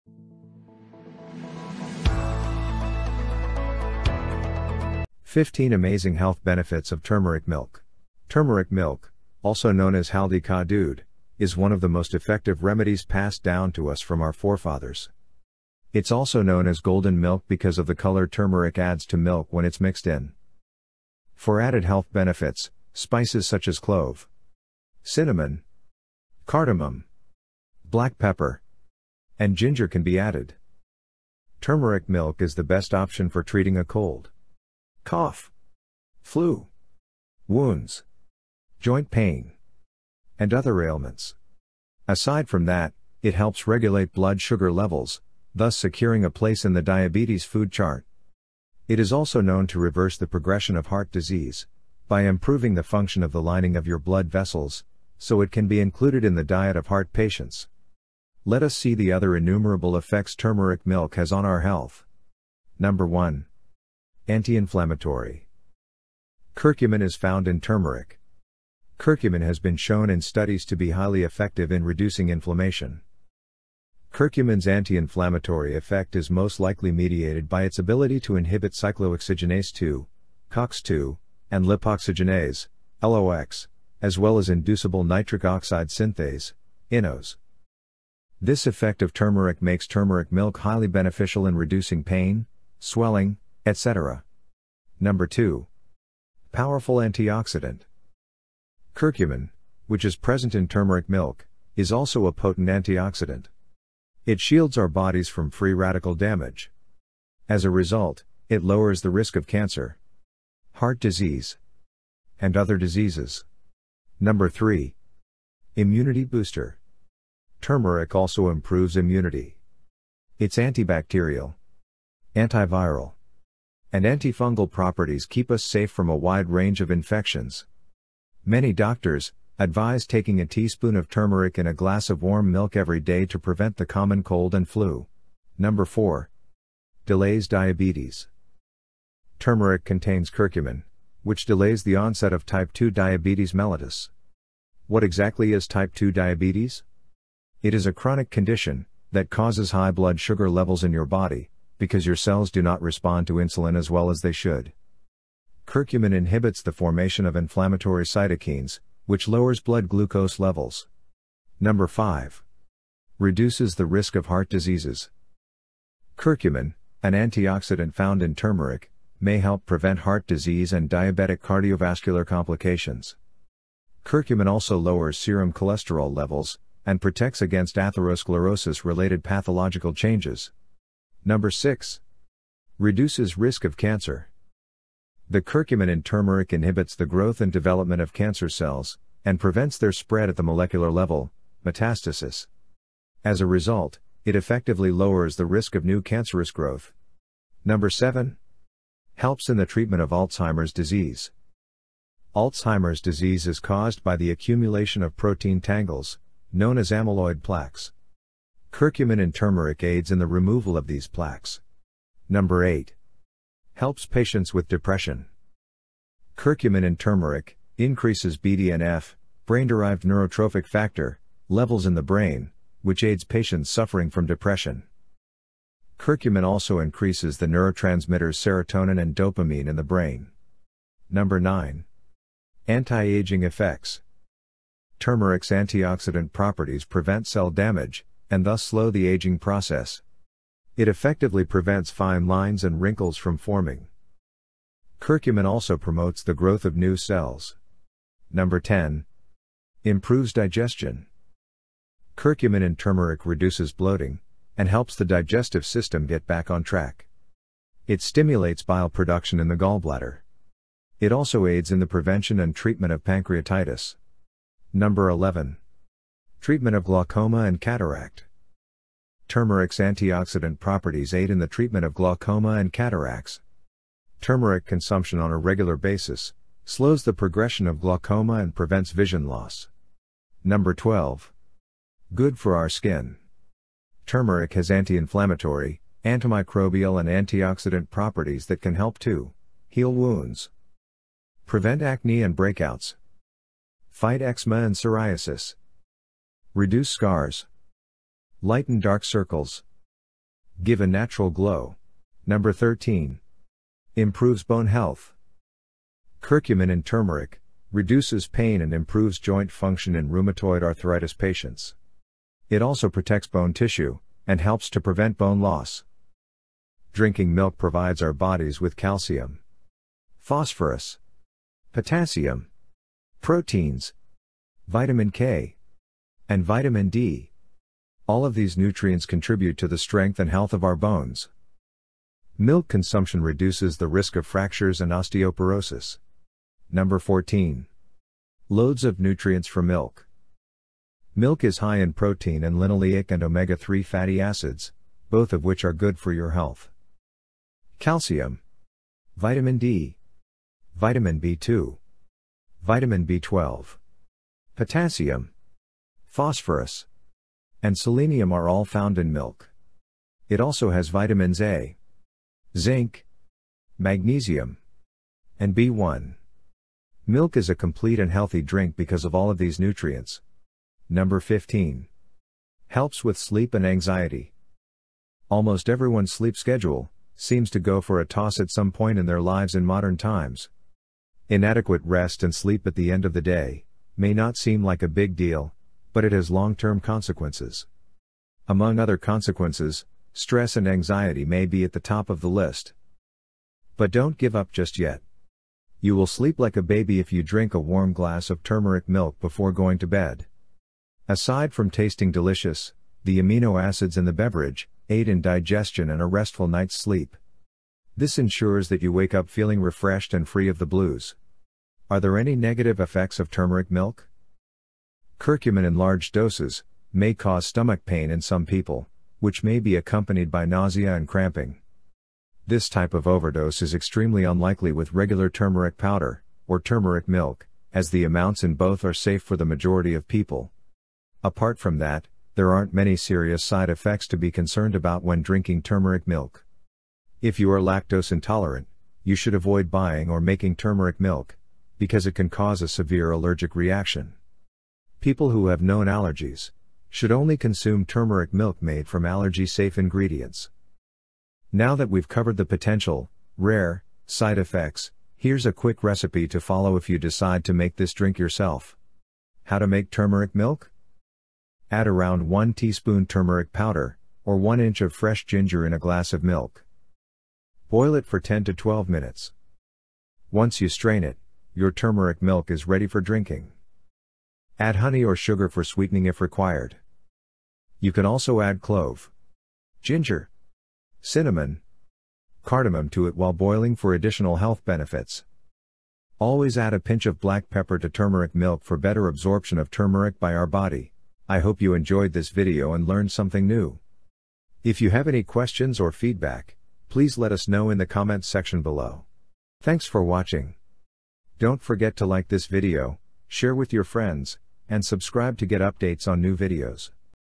1 Cup of Turmeric (GOLDEN) Milk at Bedtime...Your Body Will Thank You a Million Times
Turmeric milk is a delicious drink loaded with antioxidants that may provide an array of health benefits, ranging from a healthier brain and heart to stronger bones, improved digestion and a lower risk of disease. Curcumin is much more effectively absorbed into the body's fat and tissues when it is combined with milk.

Timestamps:
0:00 | Intro
1:02 | Anti-inflammatory
1:34 | Powerful antioxidant
1:54 | Immunity booster
2:15 | Delays diabetes
2:41 | Reduces the risk of heart diseases
3:01 | Reduces risk of cancer
3:17 | Helps in the treatment of Alzheimer’s disease
3:32 | Helps patients with depression
3:50 | Anti-ageing effects
4:08 | Improves digestion
4:24 | Treatment of glaucoma and cataract
4:40 | Good for our skin
5:02 | Improves bone health
5:36 | Loads of nutrients from milk
6:10 | Helps with sleep and anxiety
6:51 | Are there any negative effects of Turmeric Milk?
7:39 | How to make turmeric milk?

The contents of the video is not intended to be used as a substitute for professional medical diagnosis, advice, or treatment. Please seek the advice of a medical professional with all questions that you have related to or about, a medical condition.